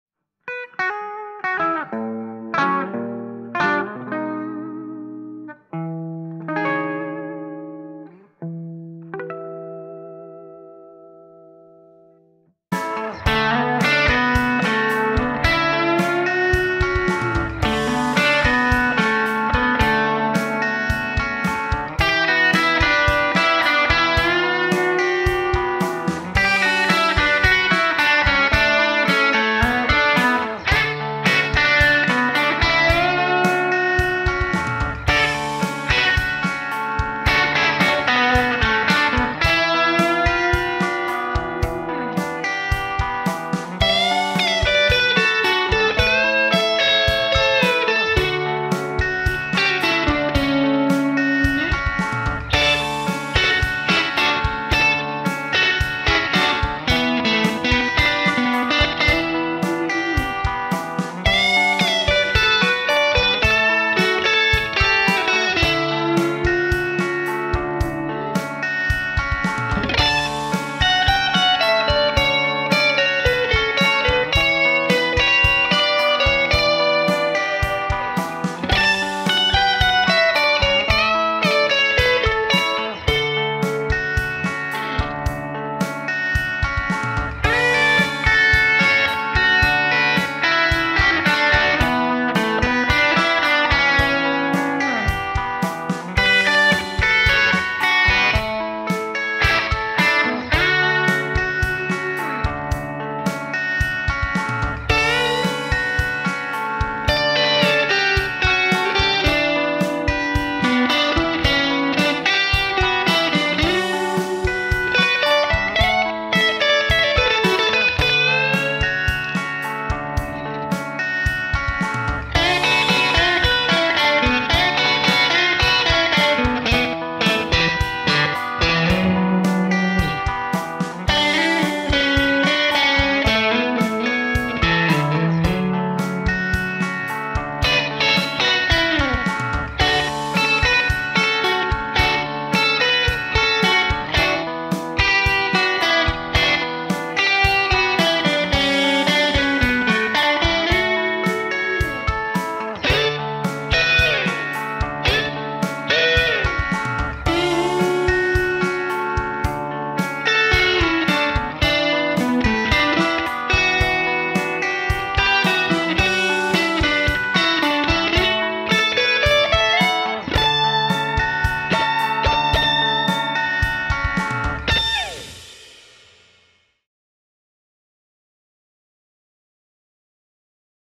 Squier Bullet Stratocaster test
Aujourd'hui, présentation de la Squier Bullet Stratocaster.

Enregistré sur Logic Pro X.

Today, presentation of the Squier Bullet Stratocaster.